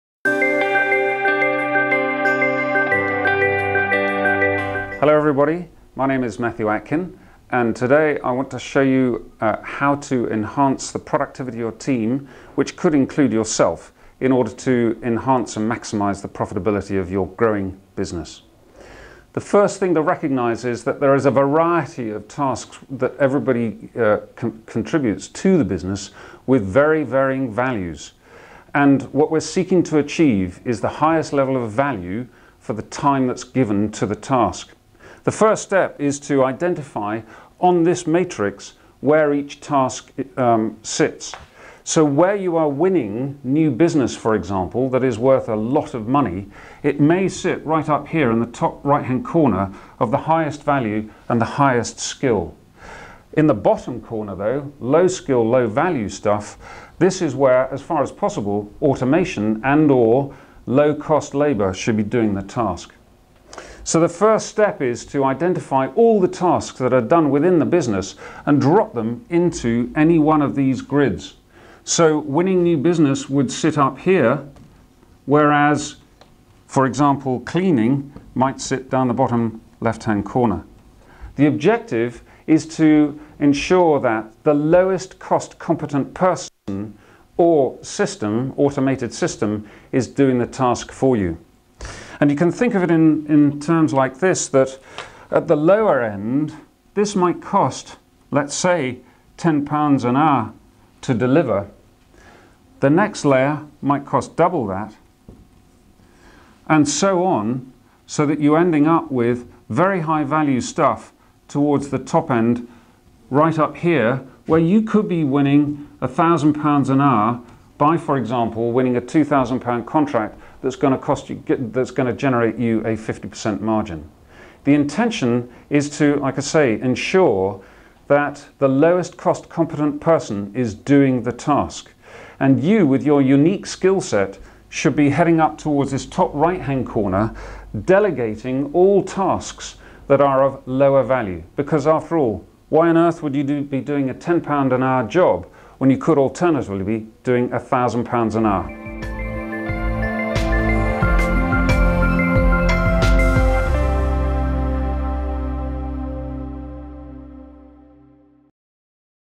Boost Your Business Productivity: Strategic Task Management for Maximum Profit
In this video, Matthew shares invaluable insights on how to supercharge your team's productivity, whether you're a business owner or an individual looking to maximise your own efficiency. Discover the key strategies to enhance your business's profitability by optimising task allocation.

Matthew delves into the concept of task valuation, emphasising that not all tasks are equal. By assigning value to each task, you can pinpoint where your efforts should be concentrated for the most significant impact. He introduces a simple matrix to categorise tasks based on their value and skill requirement.

Learn how to identify tasks that belong in the high-value, high-skill quadrant, such as winning new business opportunities. On the flip side, Matthew discusses how low-value, low-skill tasks should be automated or delegated.
Adopting this approach ensures that your business operates efficiently, with the right person or system handling each task. Matthew illustrates this concept with a cost hierarchy, demonstrating how your time should be spent on high-value activities that generate substantial profits.
Don't miss out on this practical guide to achieving optimal productivity and profitability. Start prioritising your tasks effectively today and watch your business thrive.